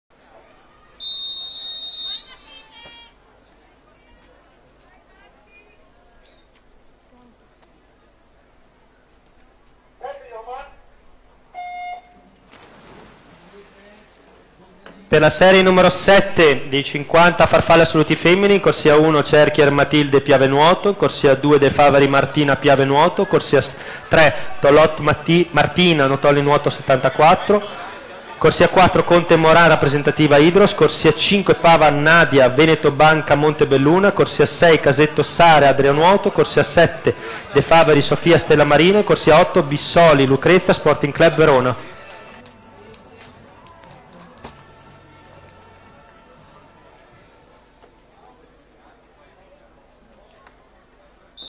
50 Farfalla Ass. Femminile Agoniste (Serie 8) - 3 Treviso Swim Cup
Atleti partecipanti:
in corsia 1 BRATTI SOFIA (2000) della soc. ARCA TEAM iscritta con 00:34.00
in corsia 2 SCAPOLAN MATILDE (2002) della soc. SPORTING CLUB NOALE iscritta con 00:33.54
in corsia 3 ZECCHINATO EMMA (2000) della soc. STILELIBERO SSD iscritta con 00:33.29
in corsia 4 BENACCHIO BENEDETTA (2002) della soc. Rappr. ROSA' iscritta con 00:33.11
in corsia 5 COVRE BENEDETTA (2002) della soc. MANIAGO NUOTO iscritta con 00:33.24
in corsia 6 BRUN ALESSANDRA (2002) della soc. PARCO LIVENZA iscritta con 00:33.50
in corsia 7 LAZZARIN ELENA (2000) della soc. ADRIA NUOTO iscritta con 00:33.79
in corsia 8 PADOIN LARA (2001) della soc. STELLA MARINA iscritta con 00:34.34